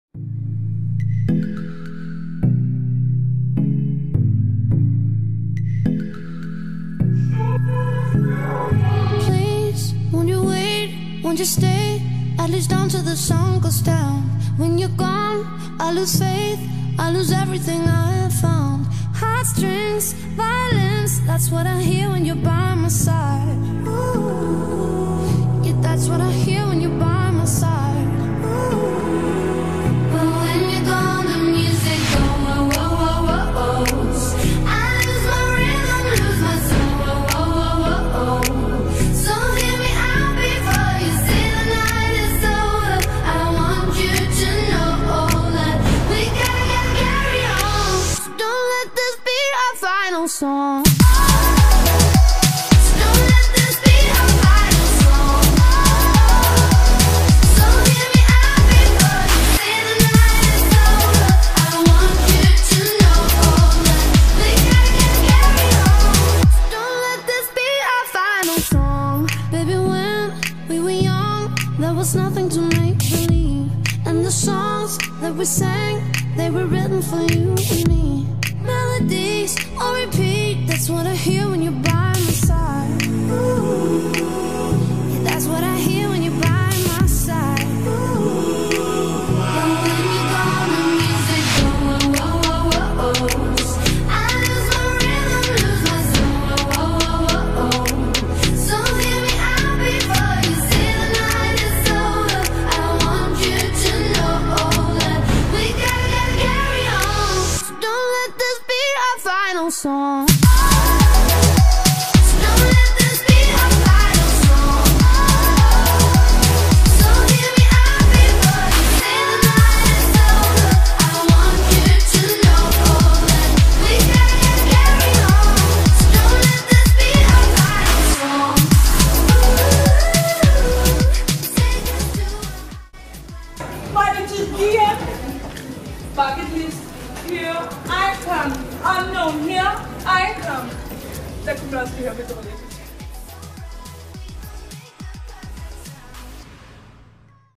Badminton på Linjetur til Kuala Lumpur, Malaysia
Video er filmet og klippet af Badmintonelev Rosa Krogh fra årgang 18/19